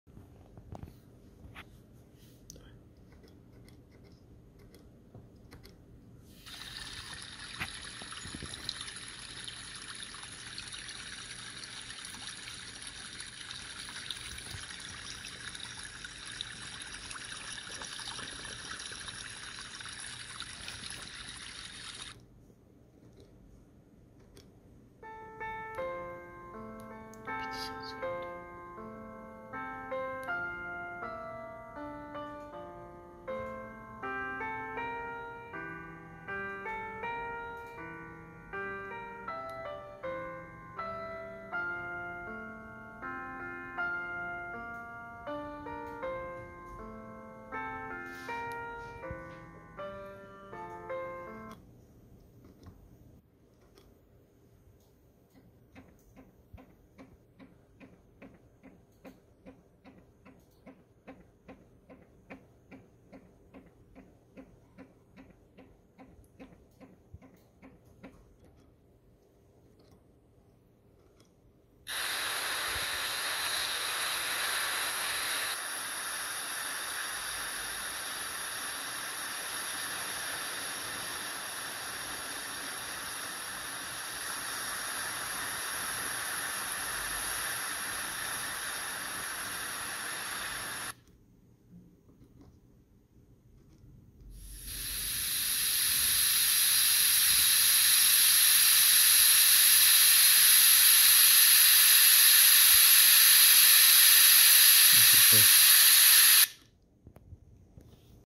Sound playback test on the ESP32 controller.
In this video you can hear 5 separate tracks being played.